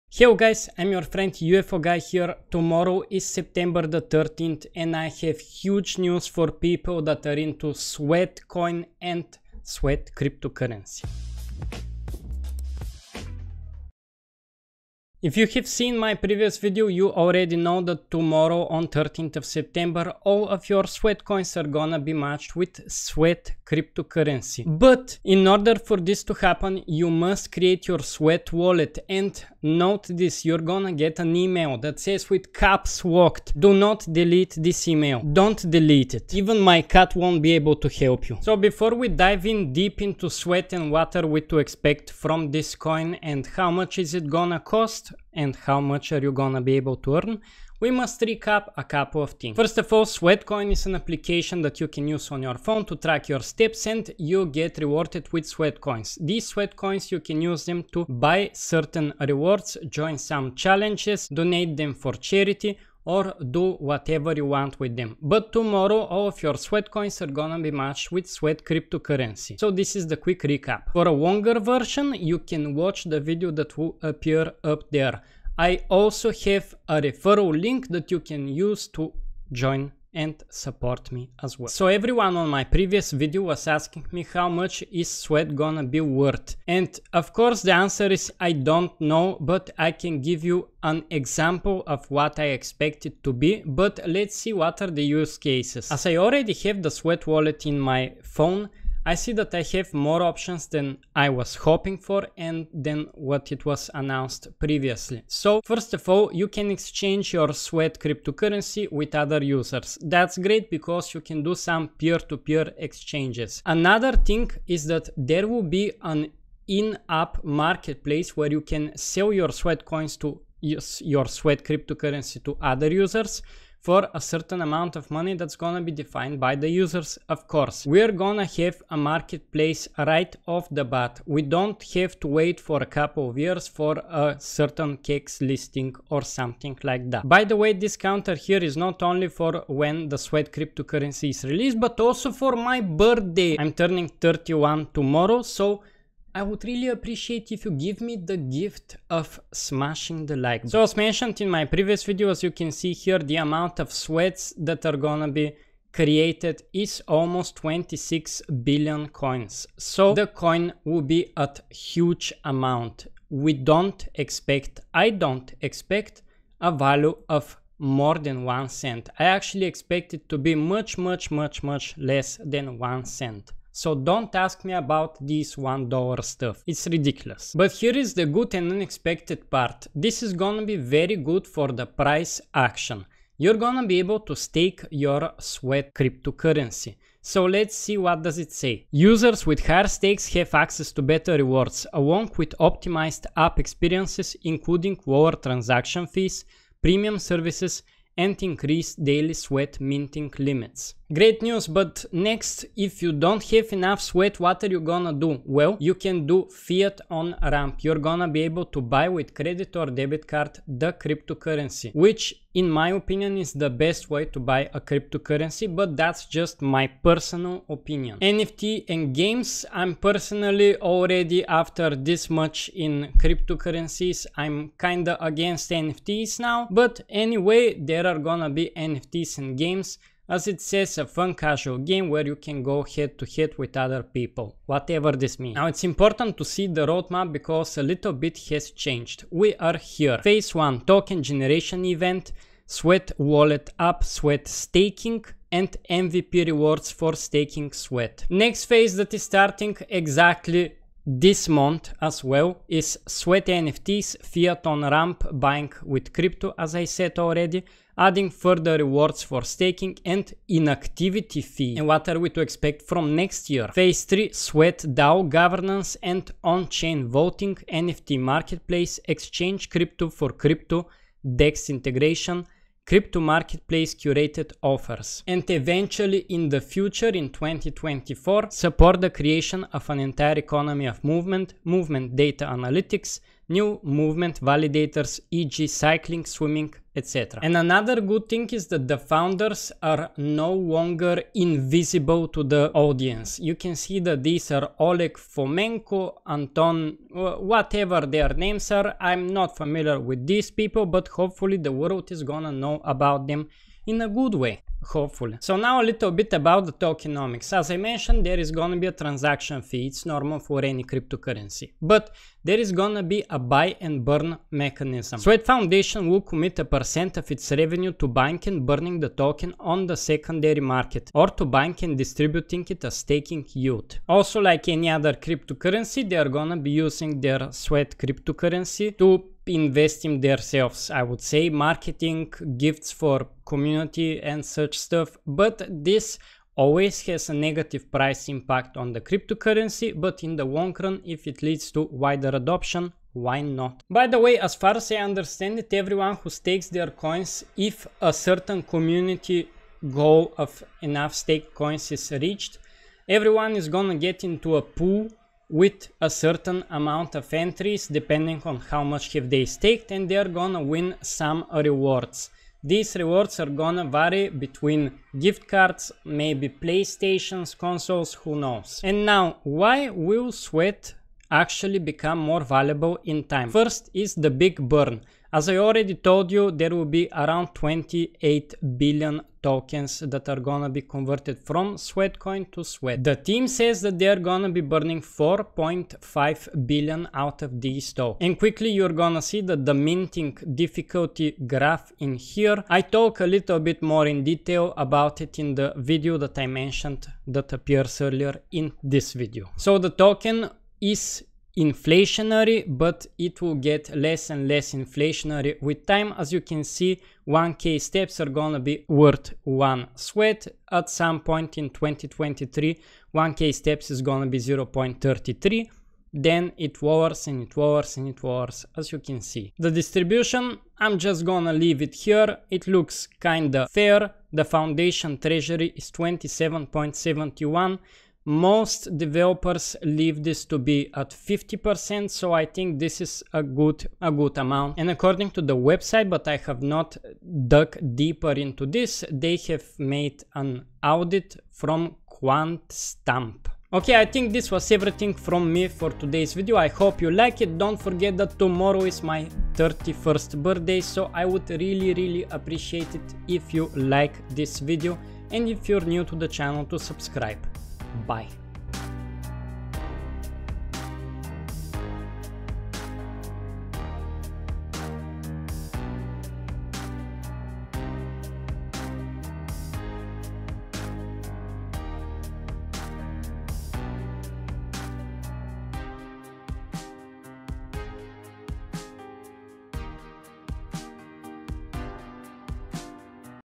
SWEAT is Here! All you need to KNOW !
Sweatcoin are introducing their cryptocurrency called SWEAT. All you need to know about SWEAT crypto ( earn by walking )

I'm not a financial advisor !
JOIN COINBASE and earn 10$ once you buy/sell 100$ worth of crypto